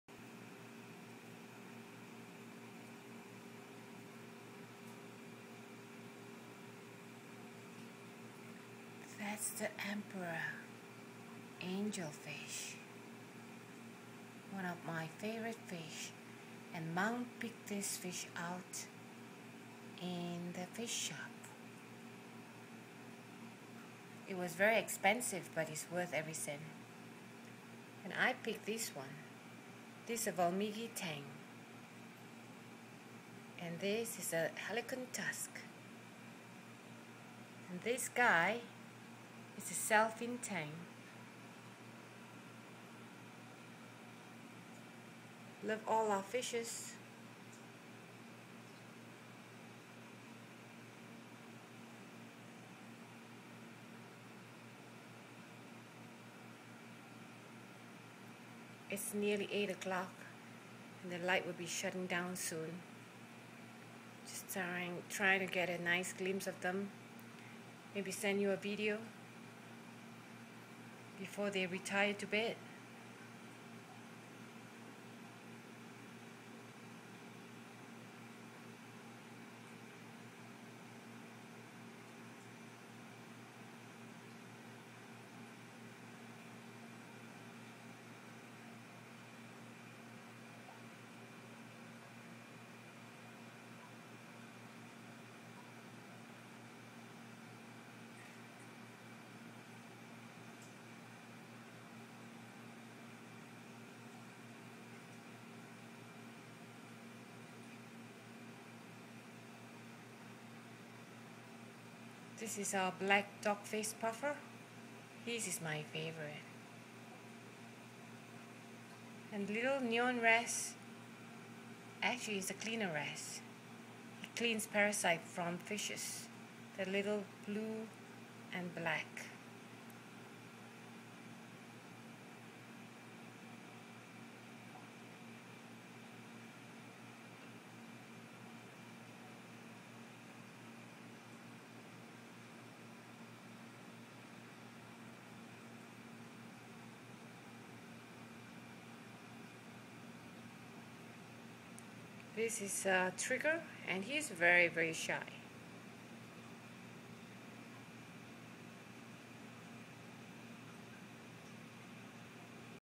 Fish swimming in our saltwater aquarium
Fishes from different parts of the ocean living in our tank and getting along very well.